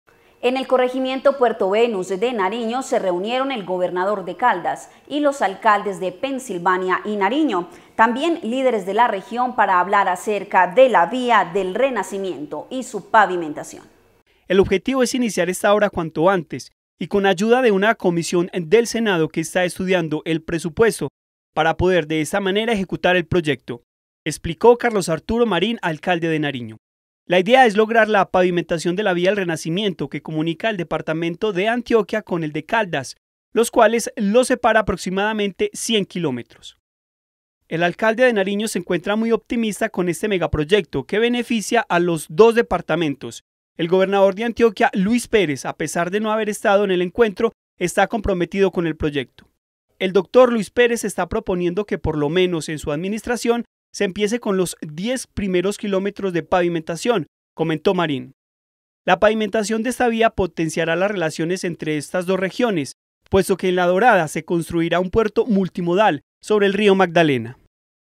REVIVE PROYECTO DE LA VÍA DE EL RENACIMIENTO
En el corregimiento Puerto Venus, de Nariño, se reunieron el Gobernador de Caldas, y los Alcaldes de Pensilvania y Nariño, entre otros líderes de la región, para hablar de la pavimentación de la vía de El Renacimiento.